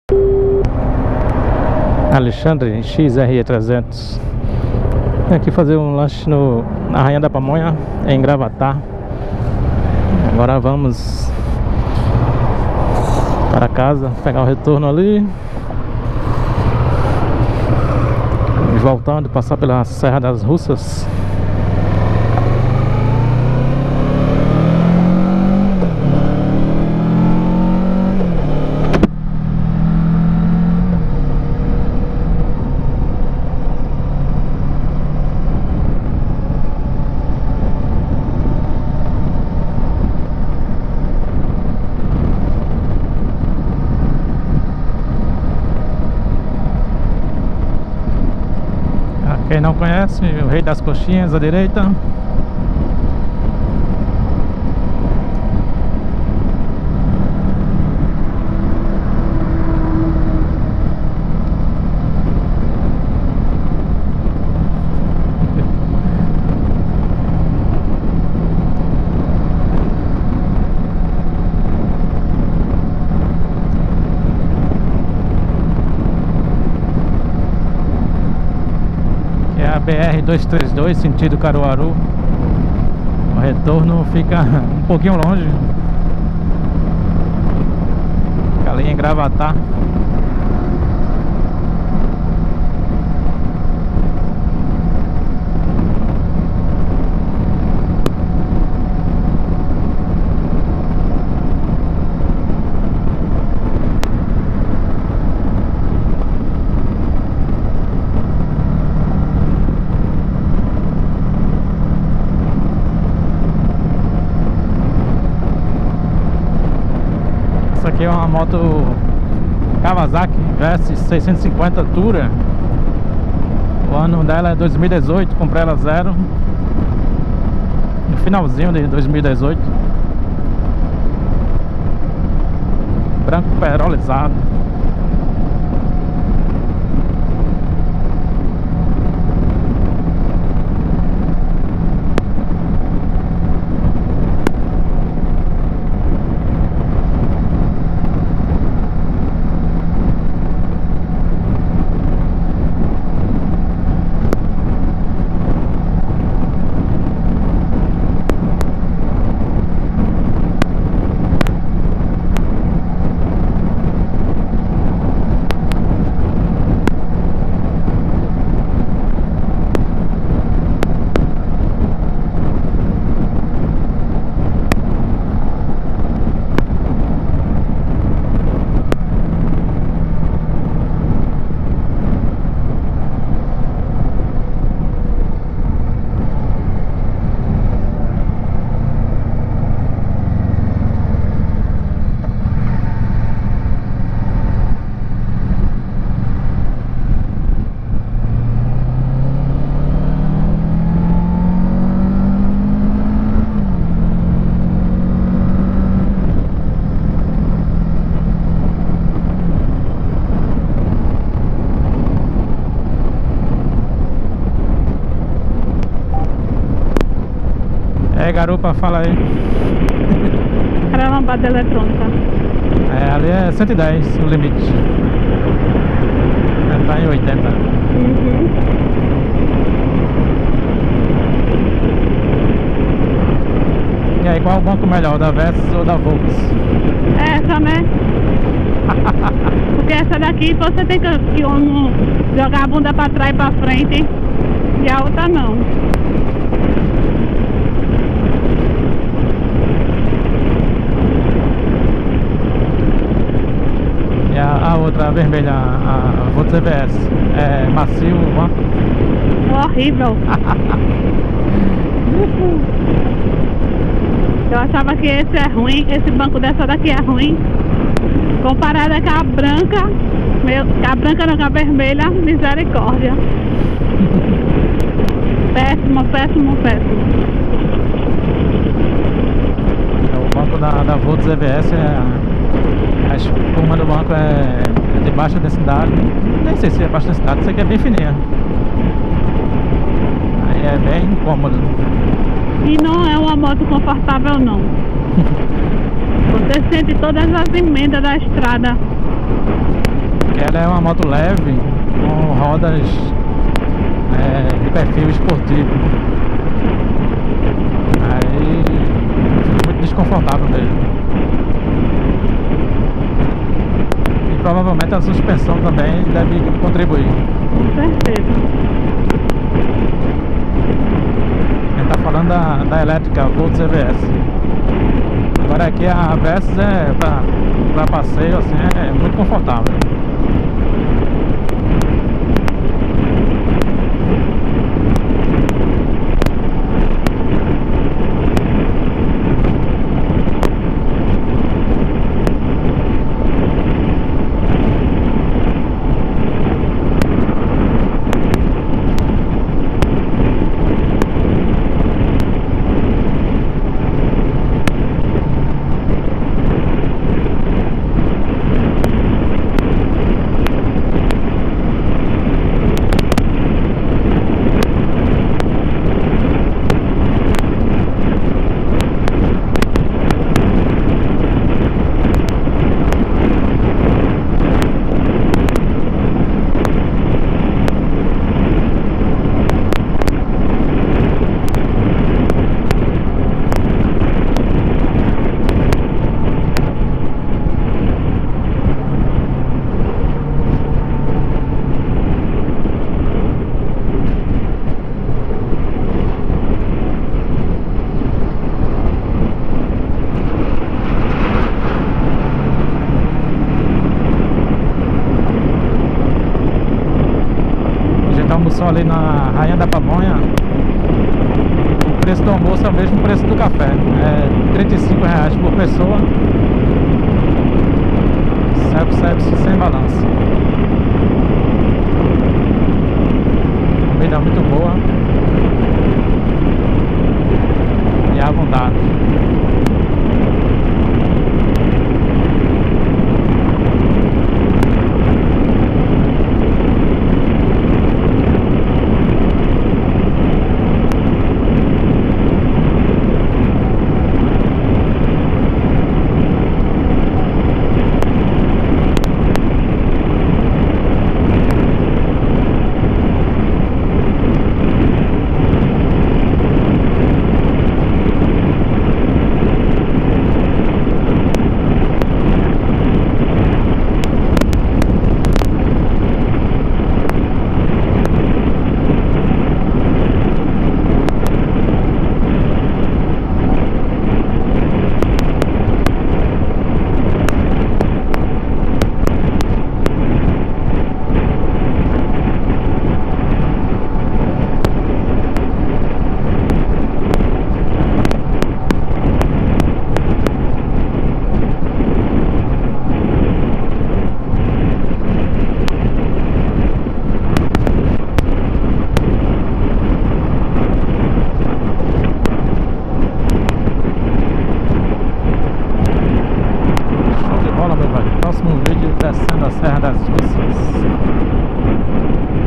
Gravatá 25/11 - VERSYS 650 ABS TOURER 2018
Resolvemos almoçar no restaurante A RAINHA DA PAMONHA em Gravatá PE onde o vídeo mostra a saída do estacionamento e o percurso pelo município.

LaranjaMecânica
Alien Motos
Tasouza Rider
Queiroz_zs